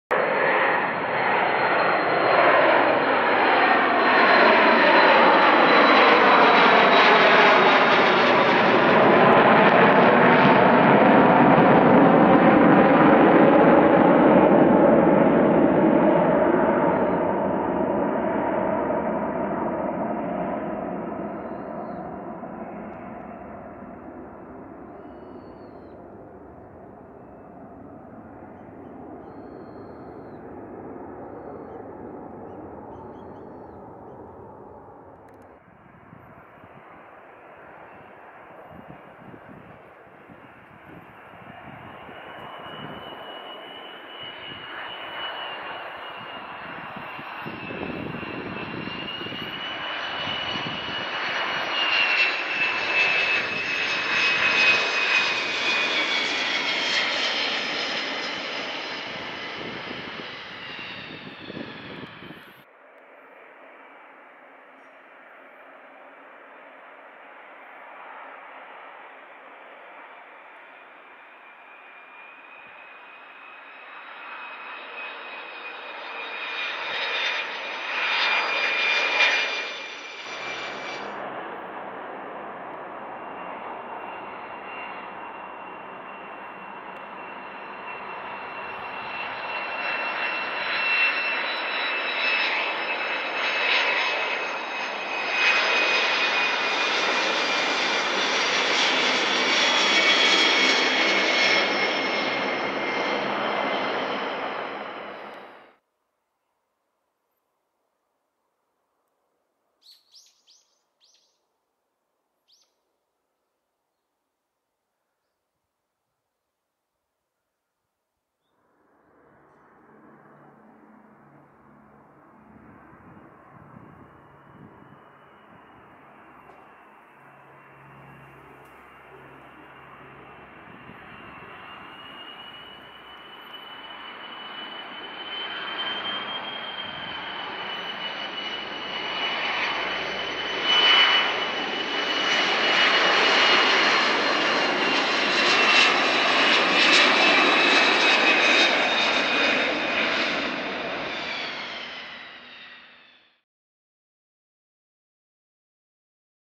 Kawasaki C-1 approach to Iruma Air Base RWY17
I often seek a good plane spotting place.
However some power pole interfere the view .
Iruma Air Base
2nd Tactical Air Lift Group 402sq / 2TAG
Japan Self Defense Force